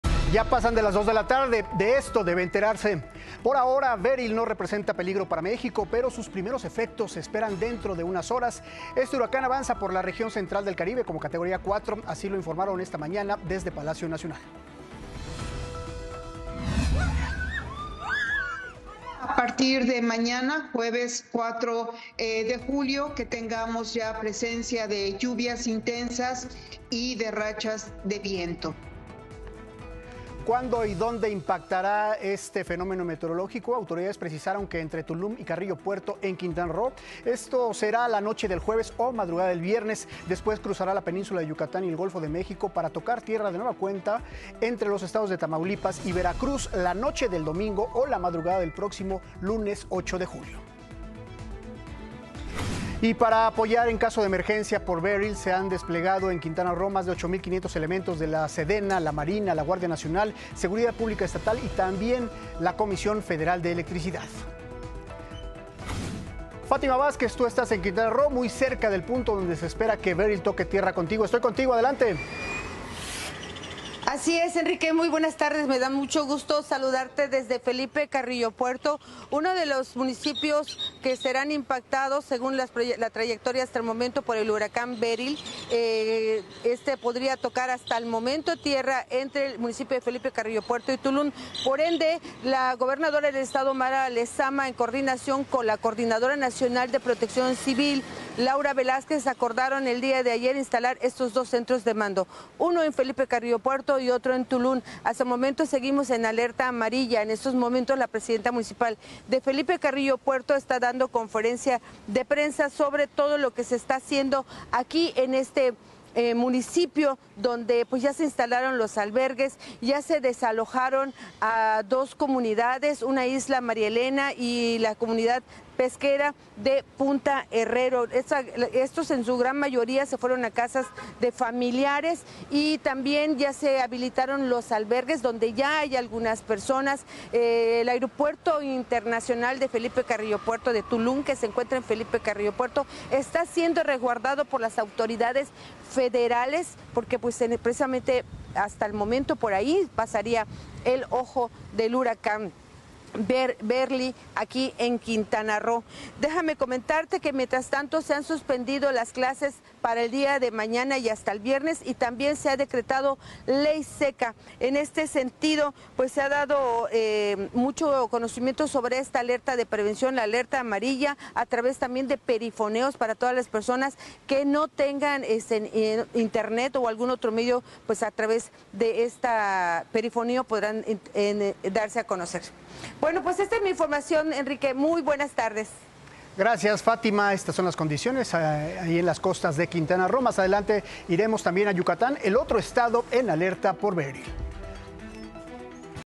El huracán “Beryl” continúa avanzando hacia México | Noticias con Crystal Mendivil
Hasta ahora Beryl no representa peligro para México, pero sus primeros efectos se esperan dentro de unas horas. Este huracán avanza por la región central del Caribe como categoría 4.